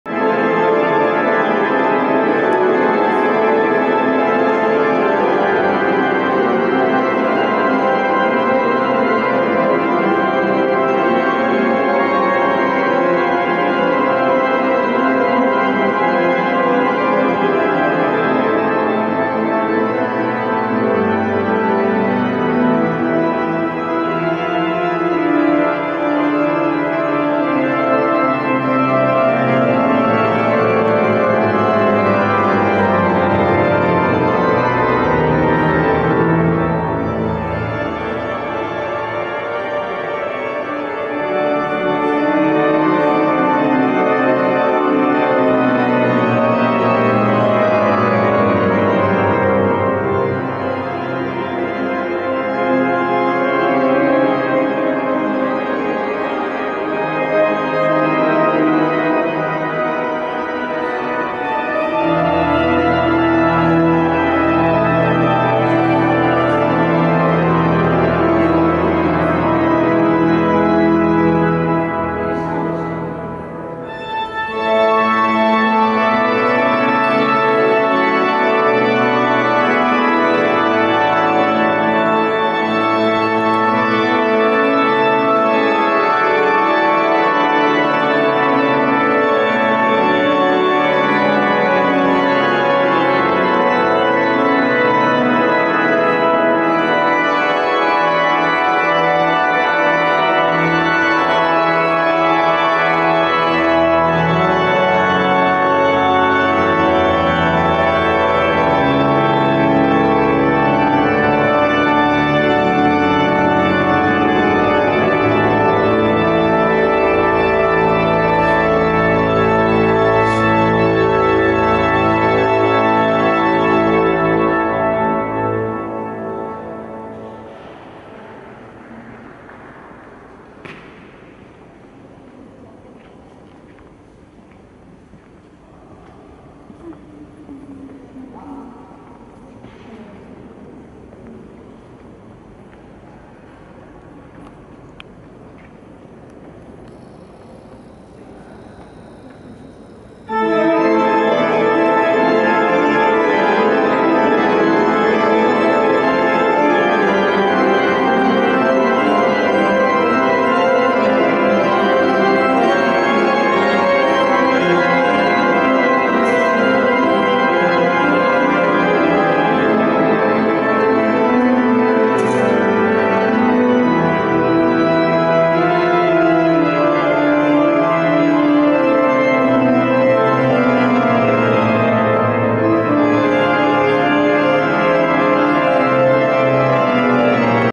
St Michael and St Gudula Cathedral, Brussels
A walk around the inside of the cathedral.